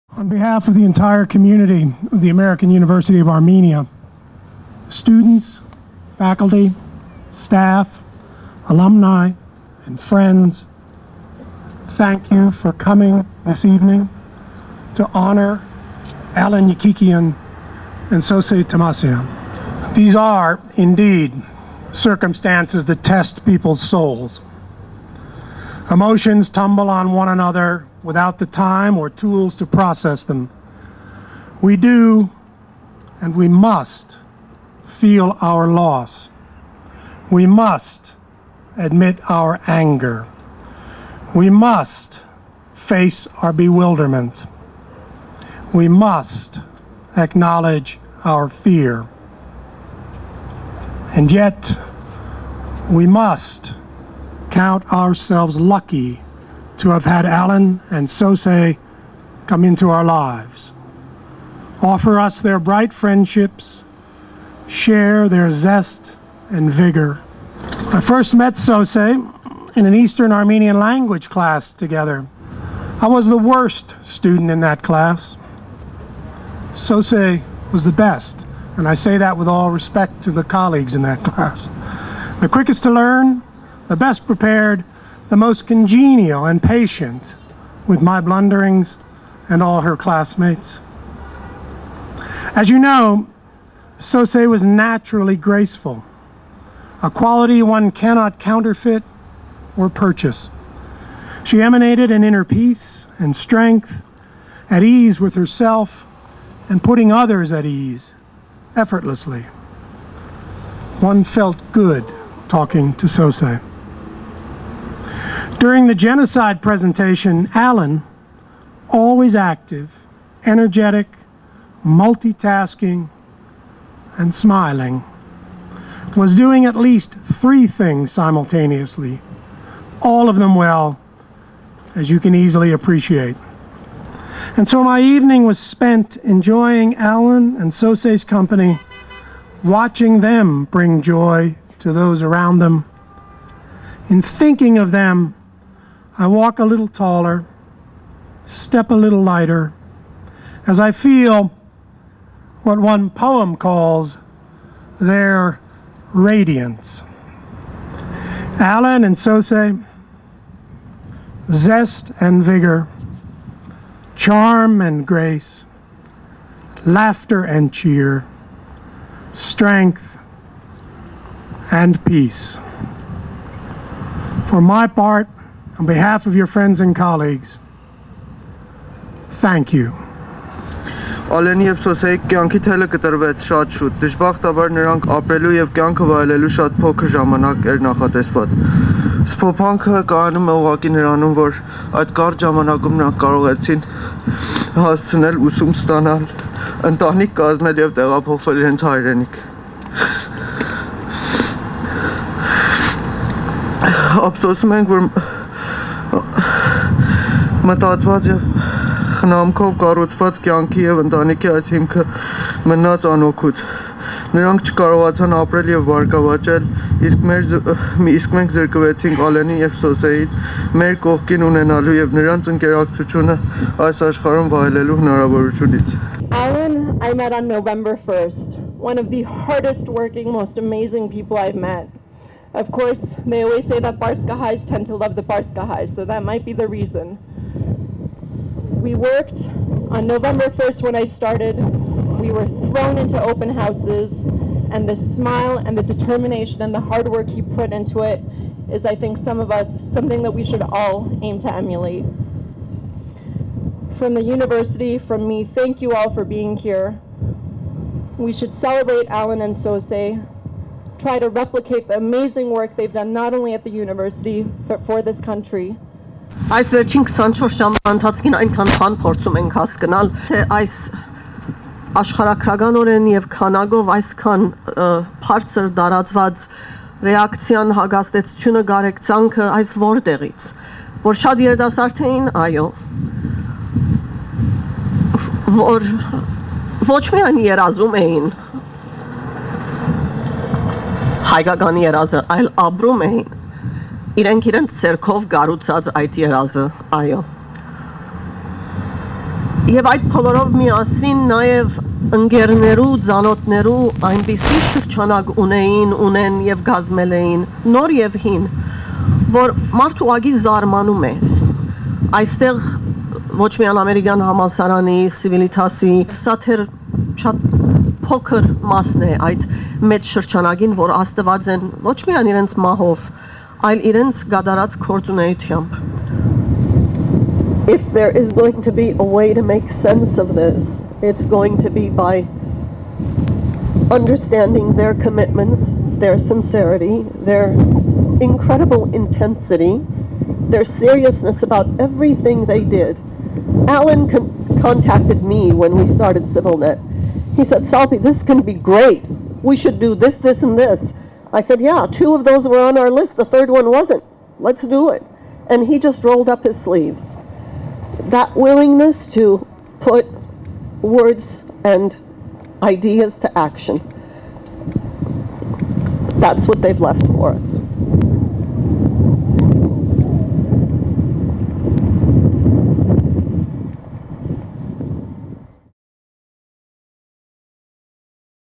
Ալենի և Սոսեի հիշատակին | Allen and Sosé. In Memoriam
Այսօր՝ մայիսի 11-ին, Հայաստանի ամերիկյան համալսարանում կազմակերպվել էր մոմավառություն՝ ի հիշատակ Ալեն Եկիկյանի և նրա կնոջ՝ Սոսե Թոմասյանի: Ալեն Եկիկյանը ՍիվիլՆեթի կայքի մշակողն ու տեխնիկական պատասխանատուն էր: Երեկոյի ընթացքում ելույթ ունեցան Ալենի և Սոսեի բարեկամներն ու գործընկերները:

Today, on May 11th, 2013, the American University of Armenia organized a candle light vigil in memory of Allen Yekikian and his wife Sosé Thomassian. Allen Yekikian was the CivilNet website developer and technical advisor. During the evening, Allen and Sosé's relatives and colleagues shared reflections about the late couple.